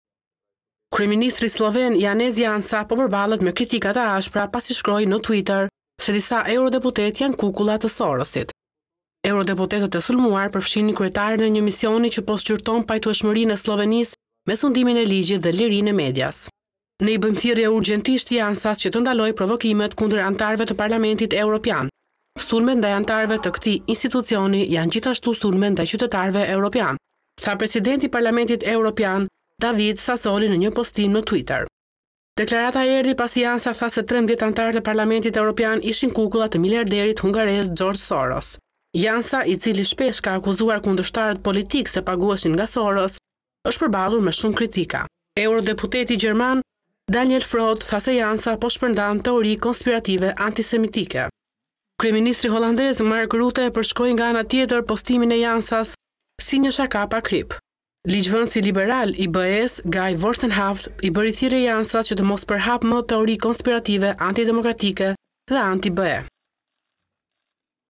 Top News-‘Janë kukulla të Sorosit’/ Kryeministri slloven sulmon Parlamentin Europian
Kryeministri Janez Janša po përballet me kritika pasi shkroi në Twitter se disa eurodeputetë janë "kukulla të Sorosit". Eurodeputetët e sulmuar përfshinin kryetarja e një misioni po shqyrton pajtueshmërinë e Sllovenisë me sundimin e ligjit dhe lirisë së medias.

“Ne i bëjmë thirrje urgjentisht Janša që të ndalojë provokimet kundër anëtarëve të Parlamentit Evropian. Sulmet ndaj anëtarëve të këtij institucioni janë gjithashtu sulme ndaj qytetarëve evropianë, ”tha Presidenti i Parlamentit Evropian David Sassoli në një postim në Tëitter.

Deklarata erdhi pasi Janša tha së 13 anëtarë të parlamentit të BE ishin kukulla të miliarderit hungarez George Soros.

Janša, i cili shpesh ka akuzuar kundërshtarët politikë se paguheshin nga Soros, është përballur me shumë kritika.

Eurodeputeti gjerman, Gjelbër, Daniel Freund tha se Janša "shpërndan teori konspirative antisemitike".

Kryeministri holandez Mark Rutte e përshkroi postim si një shaka "pa kripë".

Ligjvënësi liberal i BE-së Guy Verhofstadt i bëri thirrje Janša-s të mos përhapë teori konspirative antidemokratike dhe anti-BE.